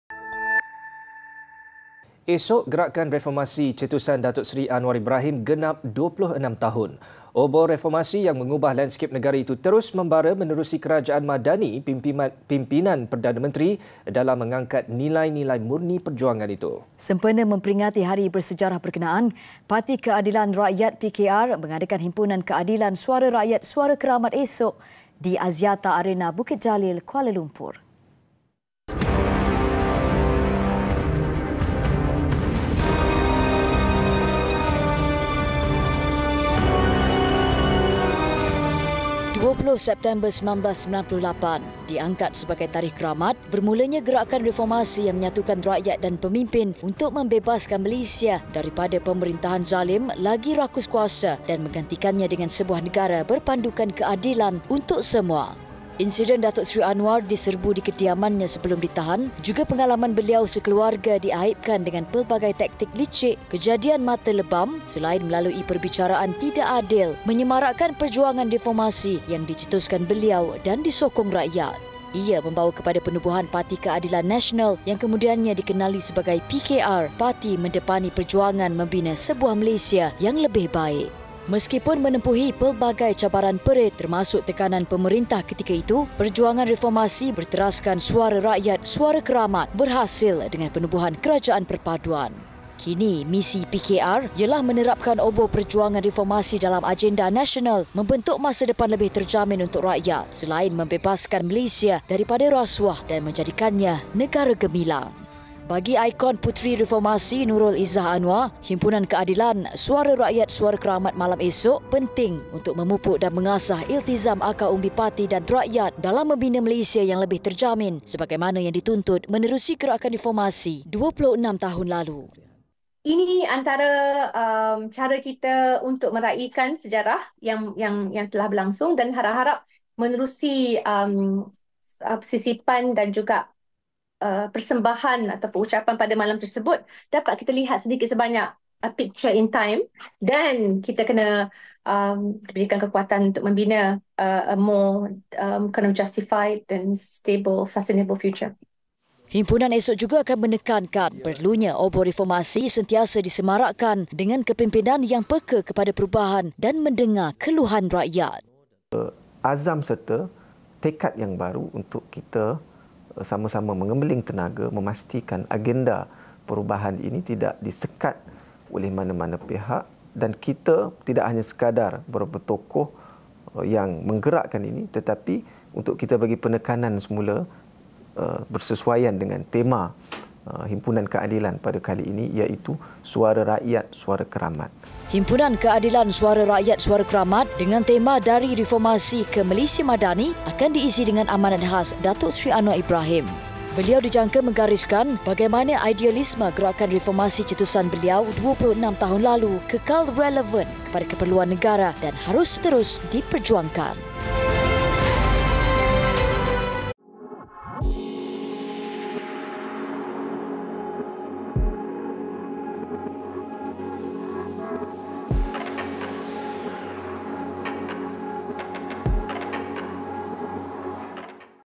HIMPUNAN KEADILAN | 26 Tahun Perjuangan, Dari Reformasi Ke Malaysia MADANI
Esok gerakan reformasi cetusan Datuk Seri Anwar Ibrahim genap 26 tahun.

Obor reformasi yang mengubah landskap negara itu terus membara menerusi Kerajaan MADANI pimpinan Perdana Menteri dalam mengangkat nilai-nilai murni perjuangan itu.